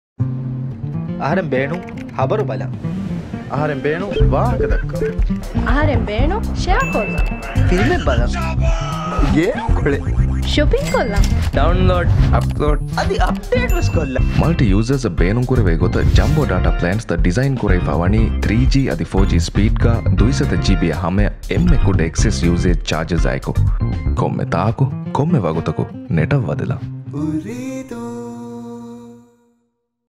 Jumbo Data
Go big with Jumbo Data plans and experience the best data speed on 3G & 4G network, with up to 200GB data allowance and lowest excess charges.